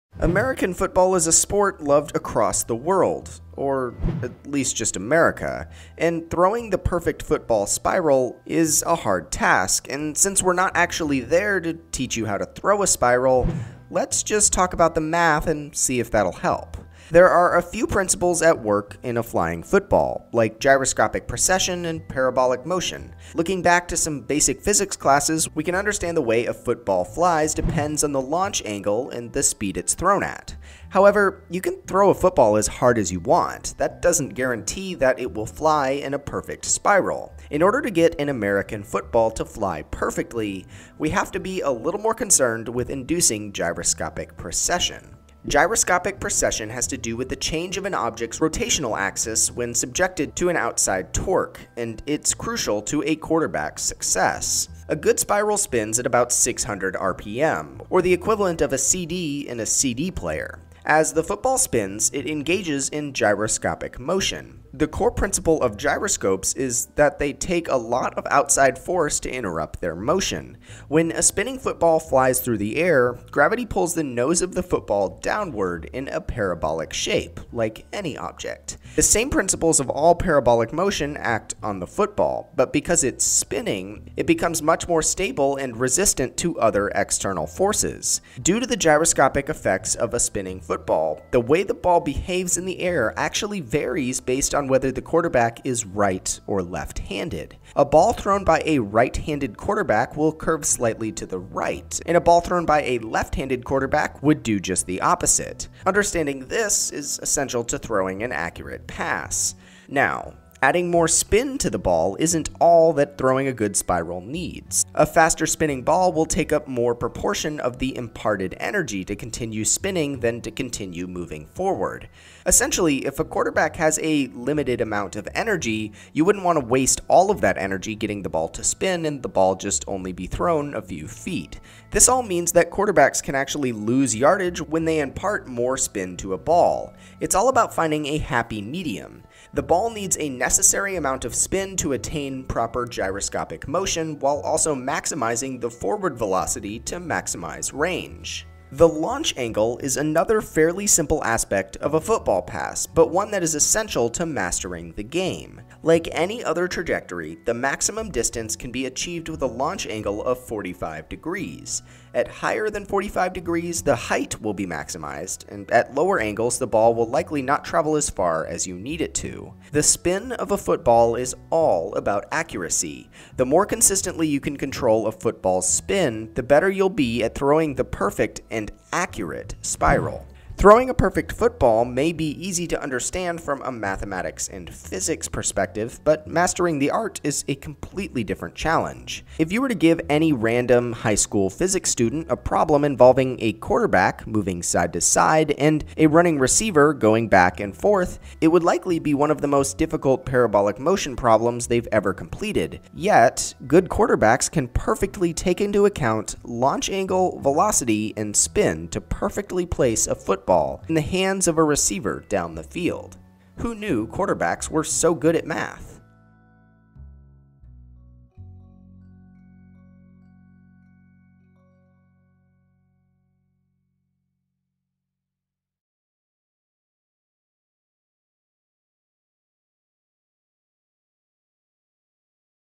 How to Throw the Perfect Football Spiral – According to Physics
Instead of learning how to throw a perfect spiral, let’s see if we can understand the physics behind a perfect football throw.

There are a few principles at work in a flying football, like gyroscopic precession and parabolic motion. Looking back to some basic physics classes, we can understand that the way a ball flies depends on the launch angle and the speed it is thrown at. However, you can throw a football as hard as you want, that doesn’t guarantee that it will fly in a perfect spiral. In order to get a football to fly perfectly, we have to be a little more concerned with inducing gyroscopic precession.